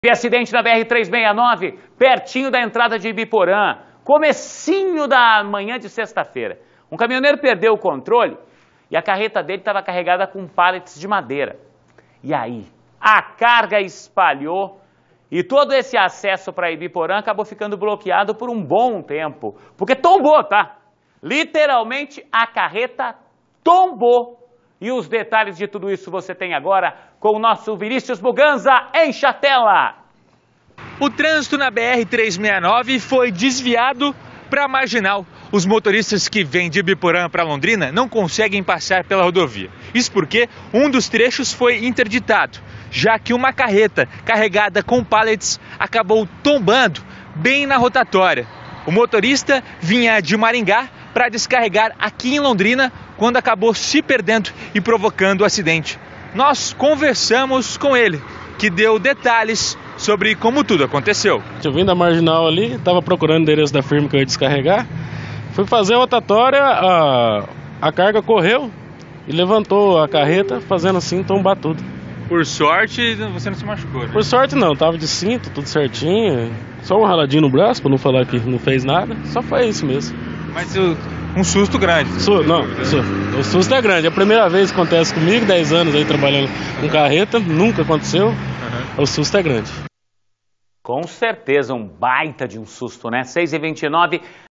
Caminhão tomba na BR-369 perto a Ibiporã
Acidente na BR-369, perto da entrada de Ibiporã, nesta sexta-feira 08/03. Um caminhoneiro perdeu o controle de uma carreta carregada com pallets. Veja mais detalhes na reportagem a seguir.